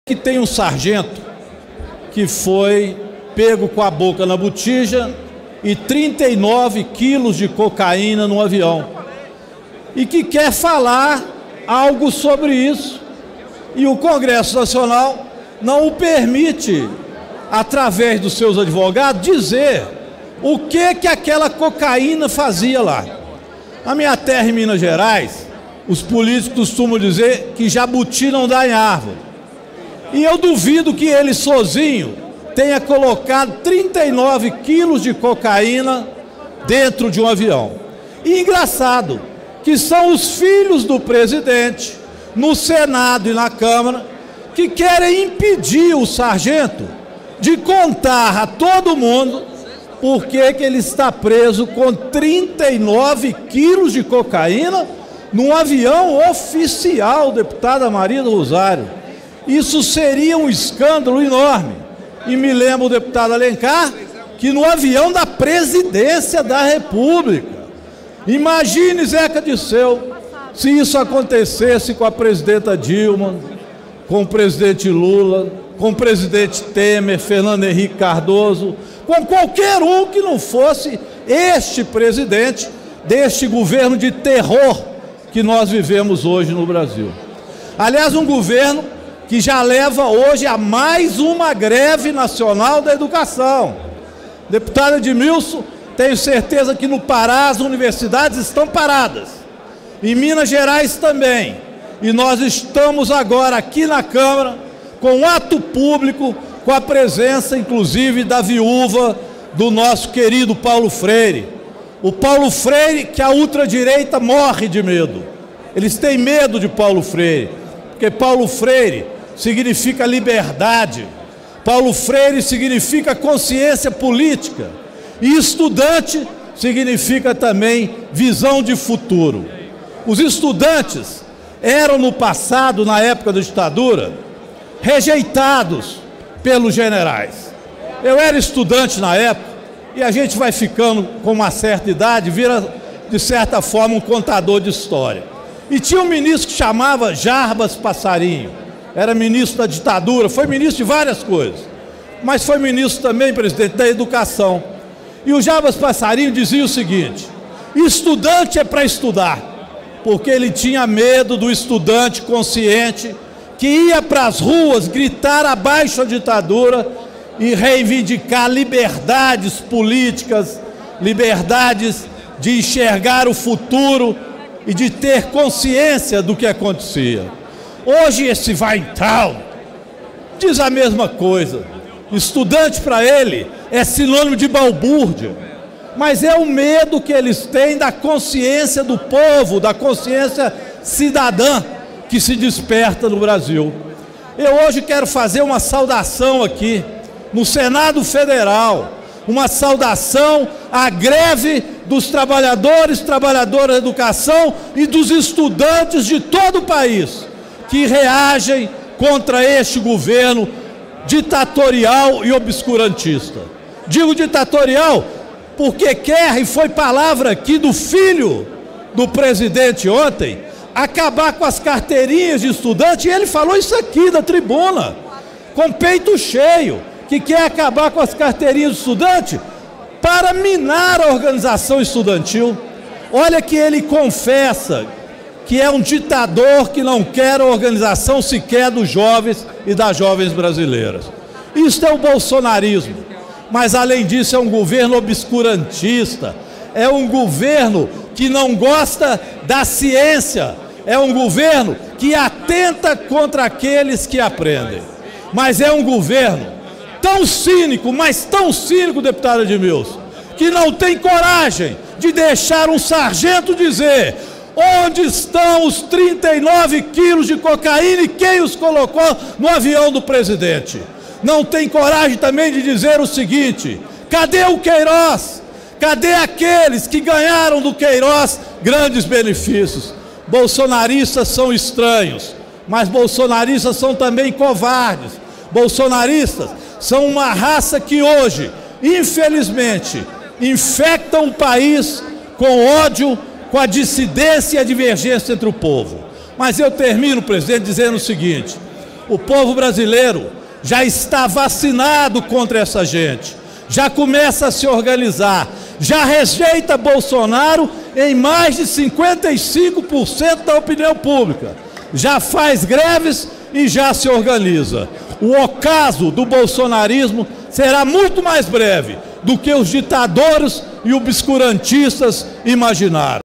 Governo morre de medo do caso do avião com cocaína
Governo Bolsonaro morre de medo do caso do avião do presidente com 39 quilos de cocaína. O sargento preso na Espanha quer falar, mas a base aliada de Bolsonaro não permite. Tá com medo por quê? O deputado federal Rogério Correia (PT-MG) fez pronunciamento sobre isso em sessão conjunta do Congresso. Confira o vídeo.